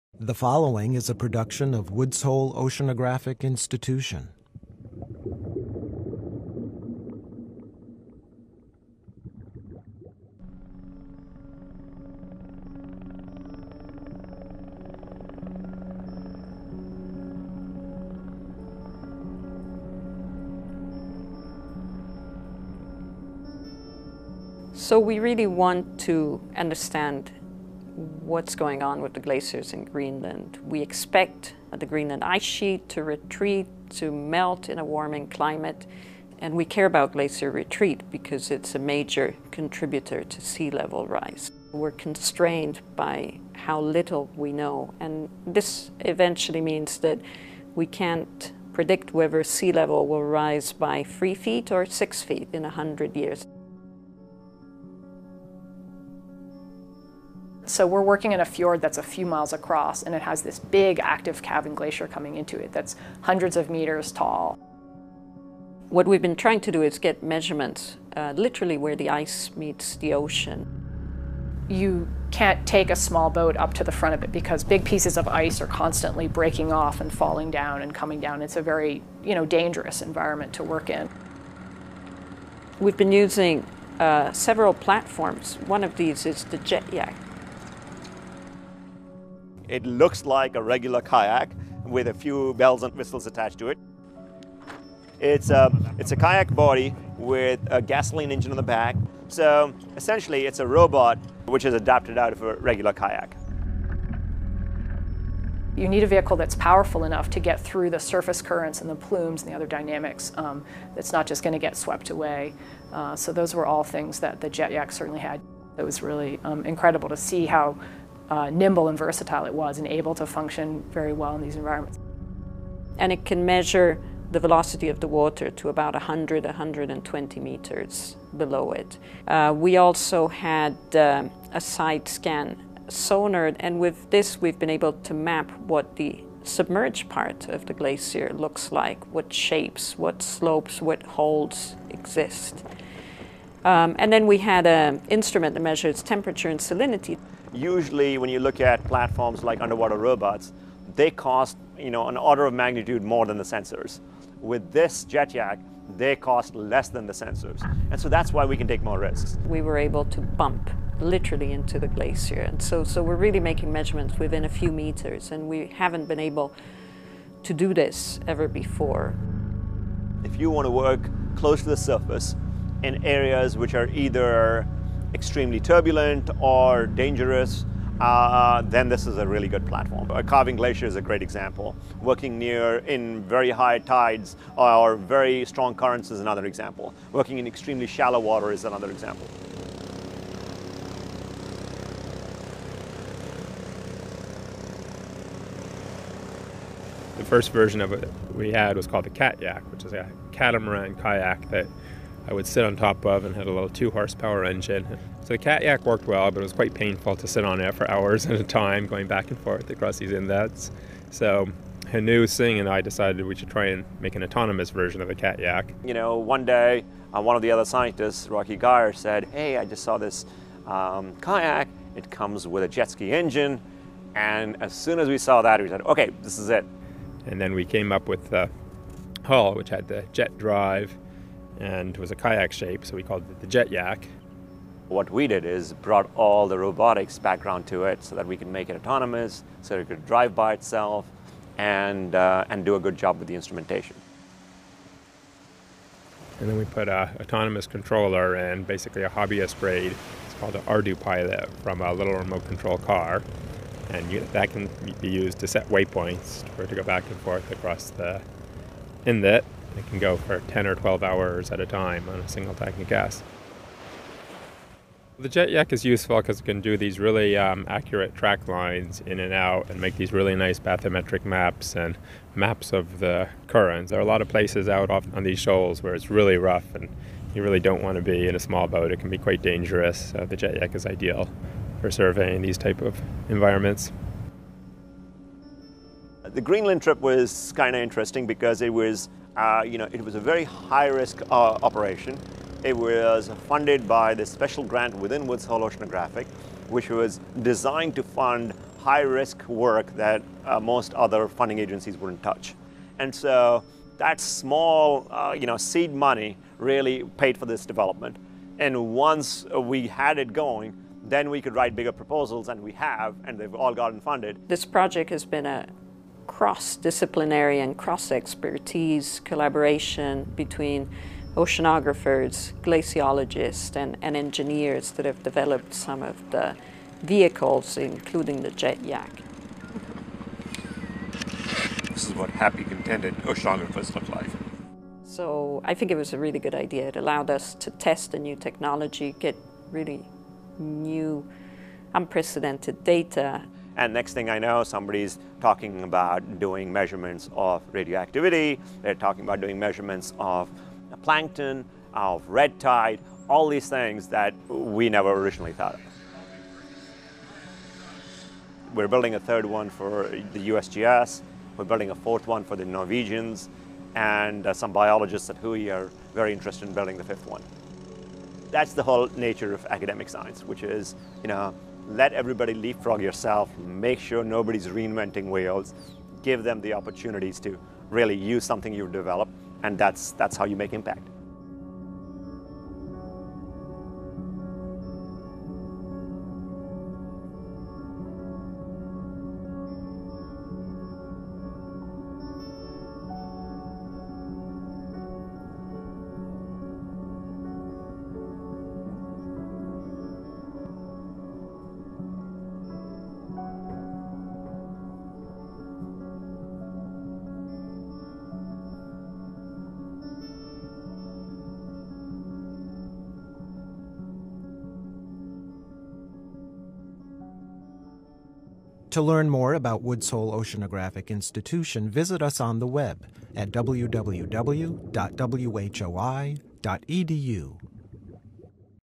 JetYak
Oceanographers are always looking for cost-effective vehicles to help them explore risky regions. Scientists at WHOI have developed one: a robotic platform called the Jetyak.